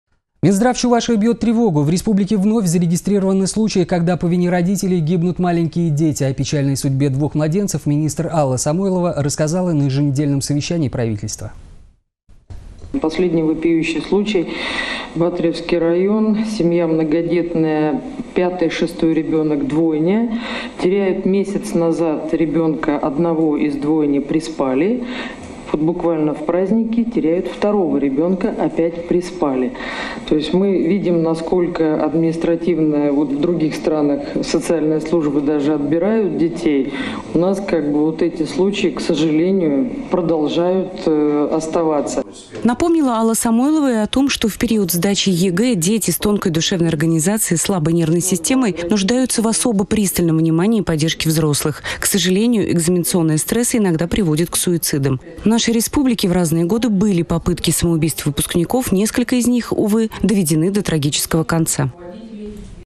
По вине родителей гибнут маленькие дети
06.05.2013
Минздрав Чувашии бьет тревогу — в республике вновь зарегистрированы случаи, когда по вине родителей гибнут маленькие дети. О печальной судьбе двух младенцев министр Алла Самойлова рассказала на еженедельном совещании правительства.